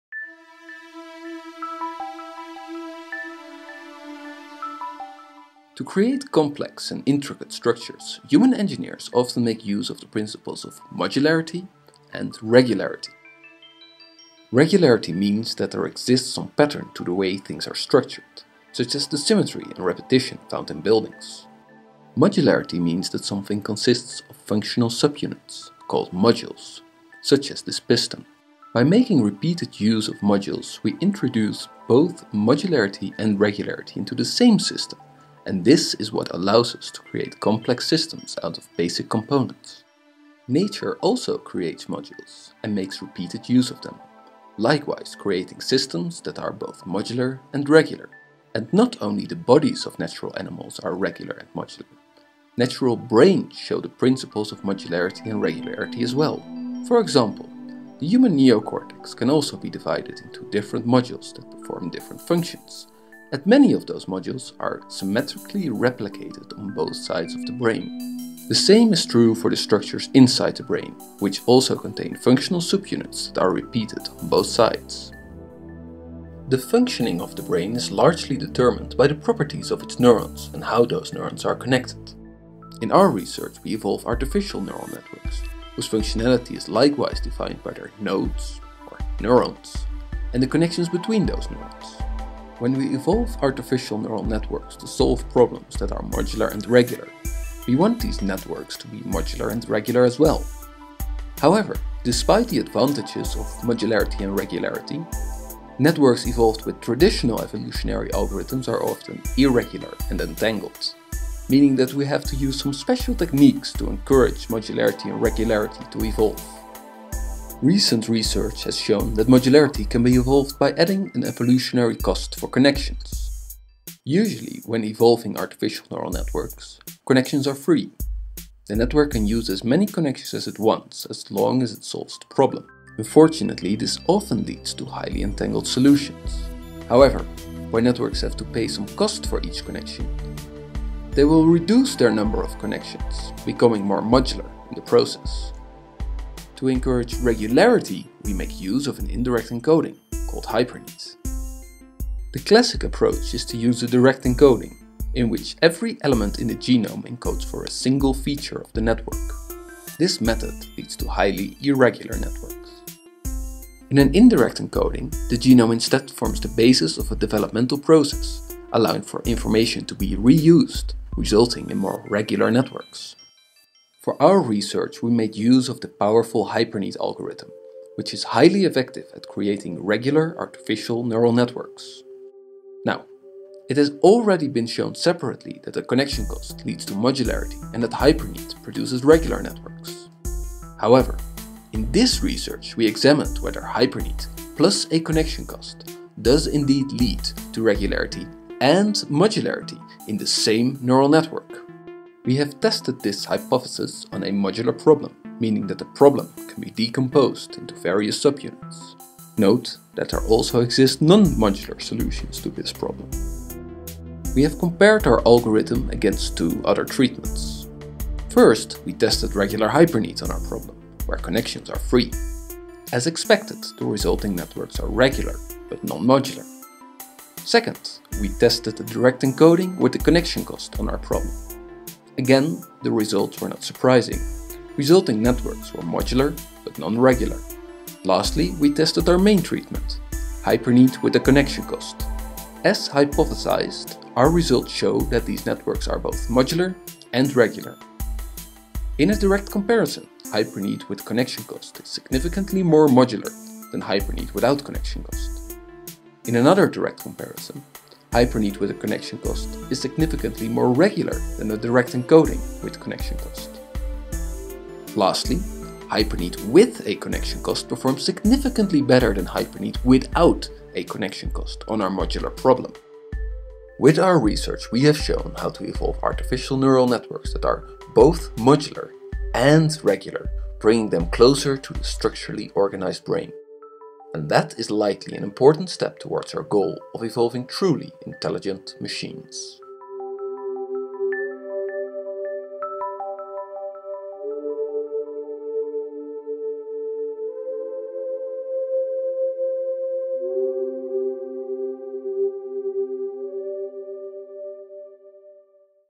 Evolving Artificial Neural Networks That Are Both Modular and Regular
A video summary of the paper: Huizinga, J, Mouret JB, Clune J (2014) Evolving Neural Networks That Are Both Modular and Regular: HyperNeat Plus the Connection Cost Technique. Proceedings of the Genetic and Evolutionary Computation Conference.

Special thanks to those who created the music, images, videos and software that were used to create this video.

Brain imaging from:
Allen Human Brain Atlas [Internet].

Animated car engine from:
Chomoka Studios